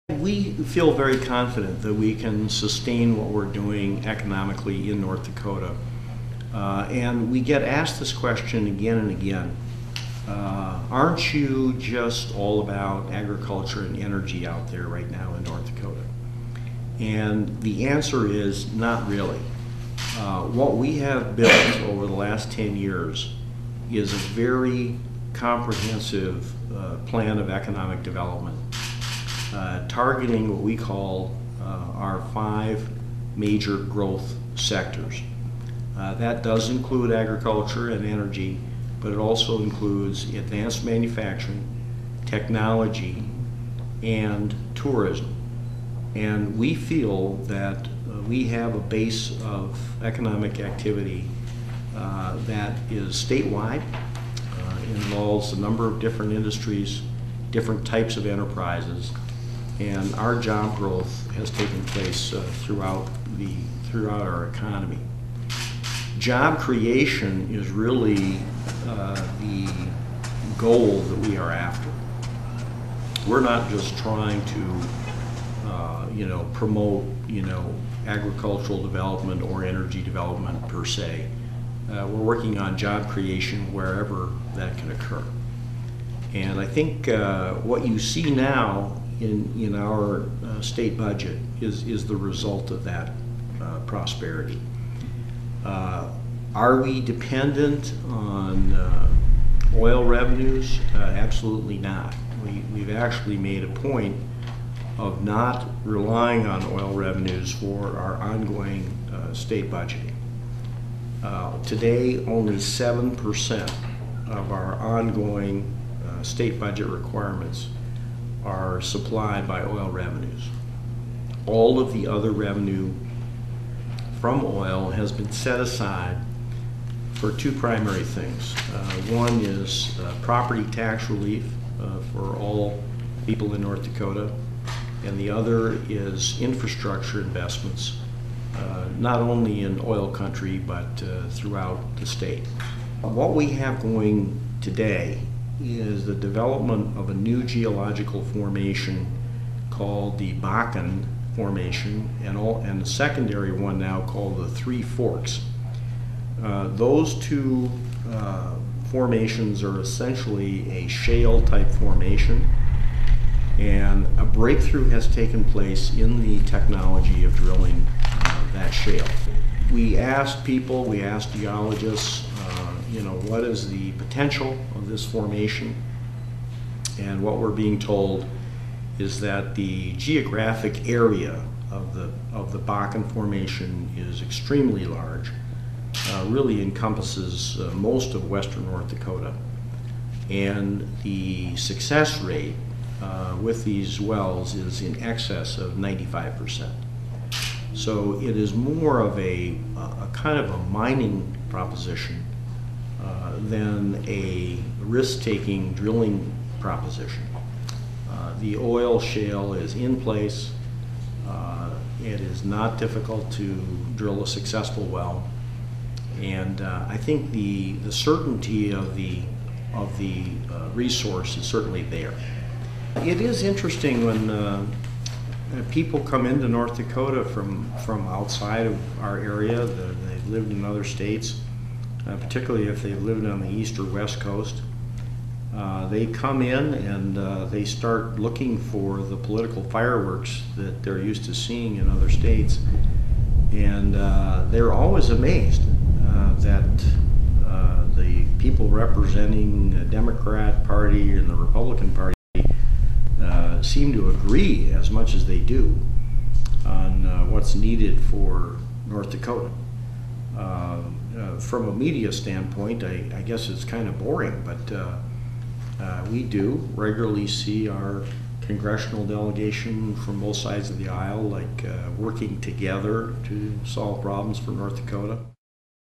Dalrymple Says North Dakota Pushing Jobs Expansion
March 23 (Bloomberg) -- North Dakota Governor John Dalrymple talks with Bloomberg's Bill Heltzel about the state's economic outlook and the growth of its oil industry. (Source: Bloomberg)